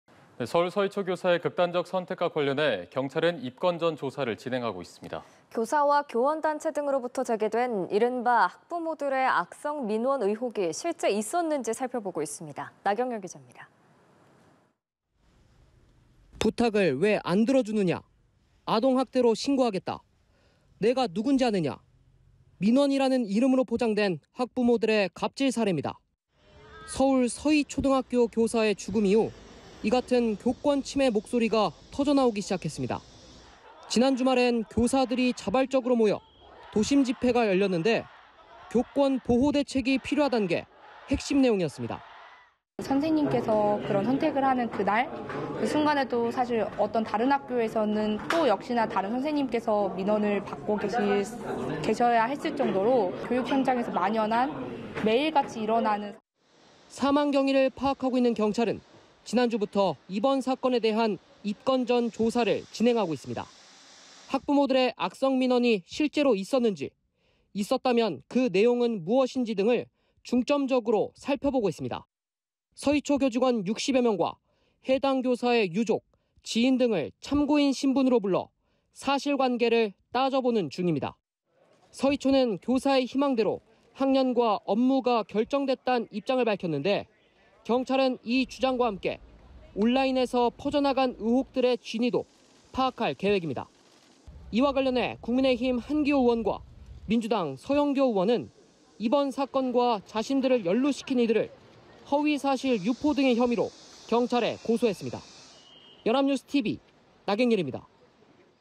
서이초 '학부모 갑질' 있었나…경찰 '진위' 조사 / 연합뉴스TV (YonhapnewsTV)
서이초 '학부모 갑질' 있었나…경찰 '진위' 조사
[뉴스리뷰]

[앵커]

서울 서이초등학교 교사의 극단적 선택과 관련해 경찰은 입건 전 조사를 진행하고 있습니다.

교사와 교원단체 등으로부터 제기된 이른바 학부모들의 '악성 민원' 의혹이 실제 있었는지 살펴보고 있습니다.

나경렬 기자입니다.

[기자]

"부탁을 왜 안 들어주느냐, 아동학대로 신고하겠다", "내가 누구인지 아느냐".

민원이라는 이름으로 포장된 학부모들의 '갑질 사례'입니다.

서울 서이초등학교 교사의 죽음 이후, 이 같은 교권 침해 목소리가 터져나오기 시작했습니다.

지난 주말엔 교사들이 자발적으로 모여 도심 집회가 열렸는데, 교권 보호 대책이 필요하다는 게 핵심 내용이었습니다.

[김희성 / 서울교사노동조합 부대변인] "선생님께서 선택을 하는 그날 그 순간에도 다른 학교에선 다른 선생님이 민원을 받고 계셔야 했을 정도로…교육 현장에서 만연한 매일 같이 일어나는…"

사망 경위를 파악하고 있는 경찰은 지난주부터 이번 사건에 대한 입건 전 조사를 진행하고 있습니다.

학부모들의 악성 민원이 실제로 있었는지, 있었다면 그 내용은 무엇인지 등을 중점적으로 살펴보고 있습니다.

서이초 교직원 60여 명과 해당 교사의 유족, 지인 등을 참고인 신분으로 불러 사실관계를 따져보는 중입니다.

서이초는 교사의 희망대로 학년과 업무가 결정됐다는 입장을 밝혔는데, 경찰은 이 주장과 함께 온라인에서 퍼져나간 의혹들의 진위도 파악할 계획입니다.

이와 관련해 국민의힘 한기호 의원과 민주당 서영교 의원은 이번 사건과 자신들을 연루시킨 이들을 허위사실 유포 등의 혐의로 경찰에 고소했습니다.